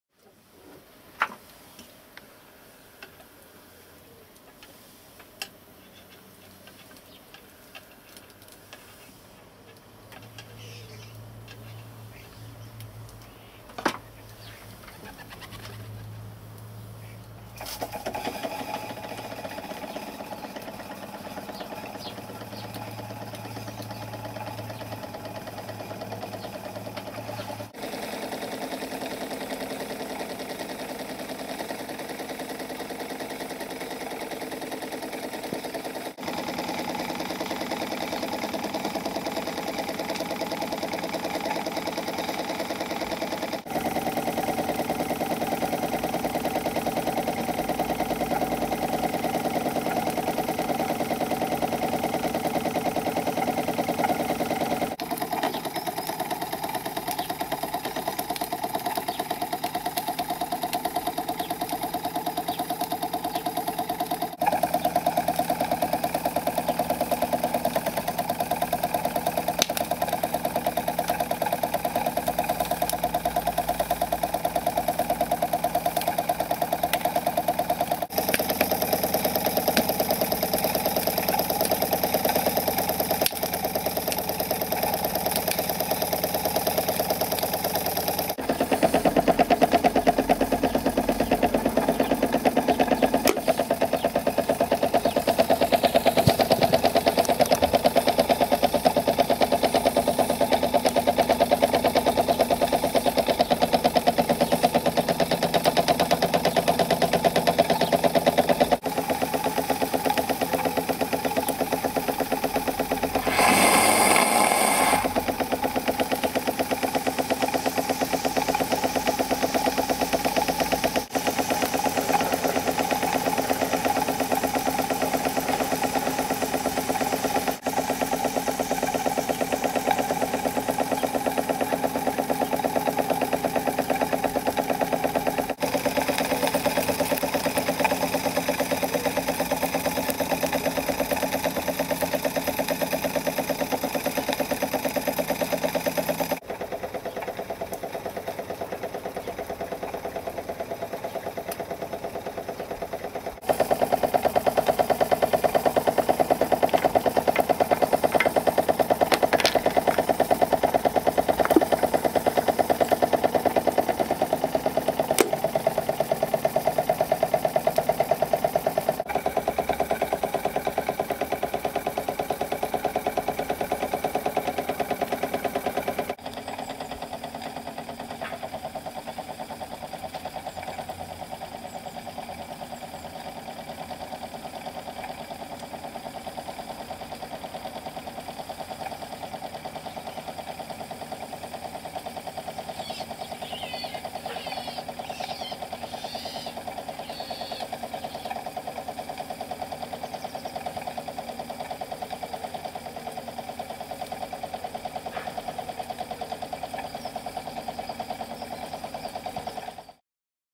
Saddle Steam Engine
First run after overhaul of this 20 year old handmade saddle steam engine